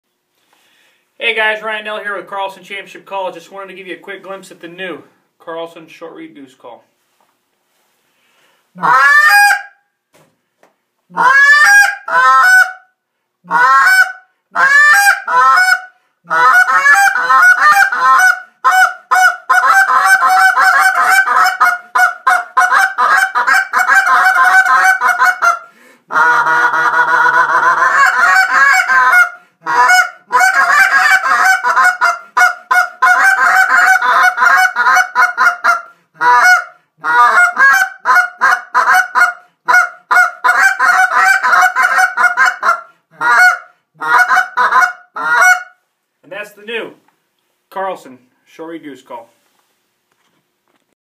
Carlson Championship Calls
The new Carlson Championship Goose Call!  Ryan Knoell out did himself this time. CarlsonCalls.com